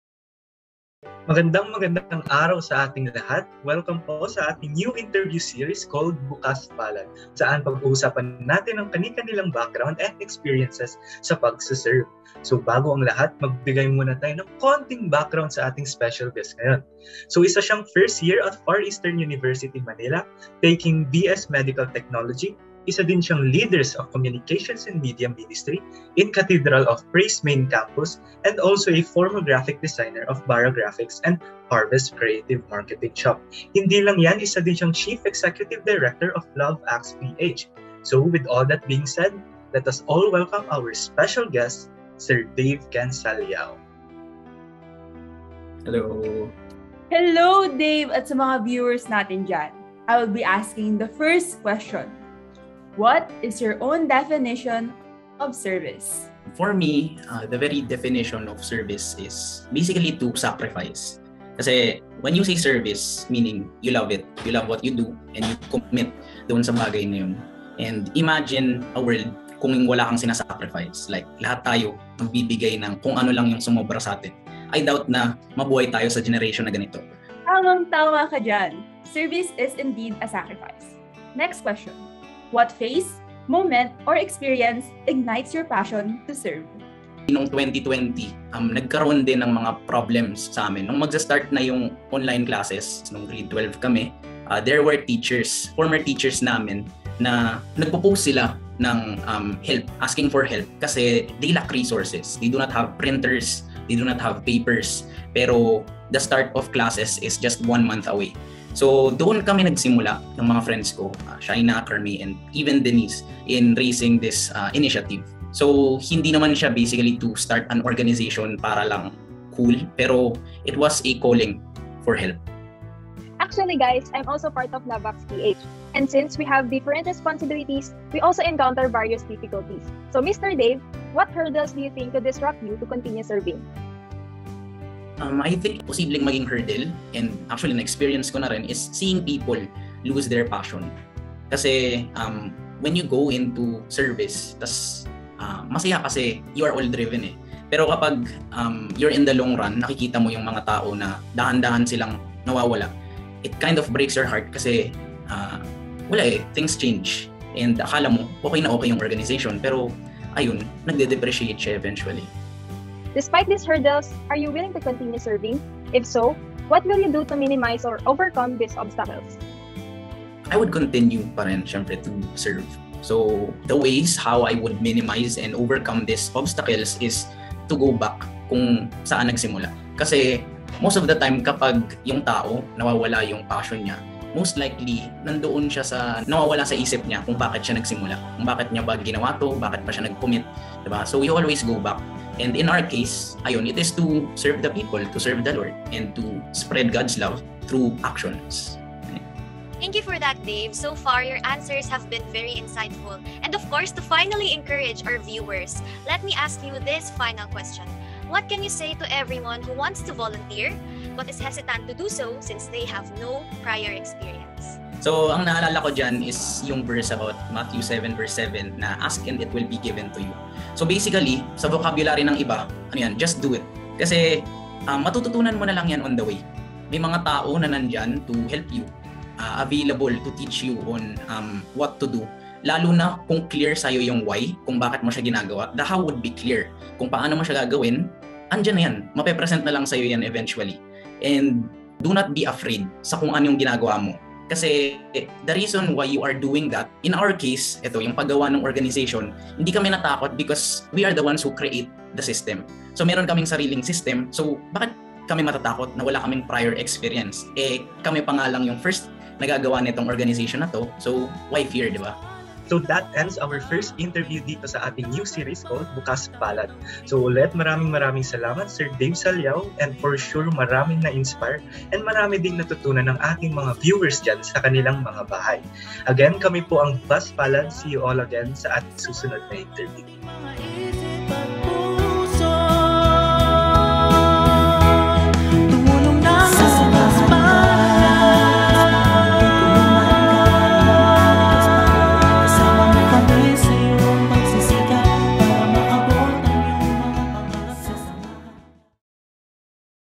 Bukas Palad: Interview Series (2021) - EP 1: LoveActs PH
Bukas Palad: Interview Series (2021) - A Short Interview Series about Zeal for Service
Episode 1: LoveActs PH
Starring: Andrei Acosta, Eli Gotoc, Denise Mangasim, Jedi Santos
Special Guest: Mr. Dave Ken G. Sallao (Chief Executive Director, LoveActs PH)

Editor: Justin Carl H. Dela Rosa
Writers: Kyle Anthony M. Aposaga, Maria Mikaela P. Leopoldo, Pia Maxine G. Mariano, Cherry Ann B. Templo

This episode was recorded on December 8, 2021.